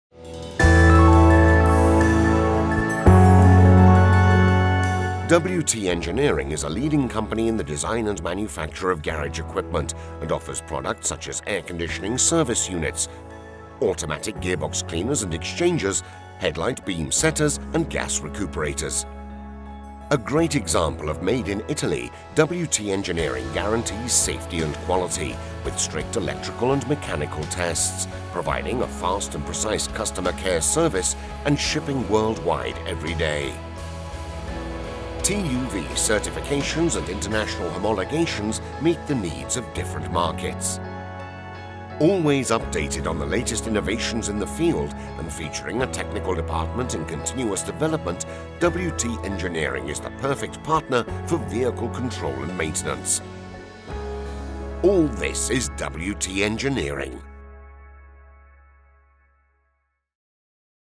W.T. ENGINEERING - Corporate video (short version) - English
W.T. Engineering s.r.l is a leader company in the market of design and construction of garage equipment thanks to its operational and design experience. We are specialists in the production of air conditioning charging units for cars, buses, trucks and tractors. In our company we produce five different models of headlight testers (UTAC, VOSA, MCTC approved), gas recovery systems, sanitation and charging station for passenger compartments, paint drying lamps and automatic gears washing machines.
Our headquarter, offices, showroom and production department are in Gandino , Seriana valley in the Bergamo area.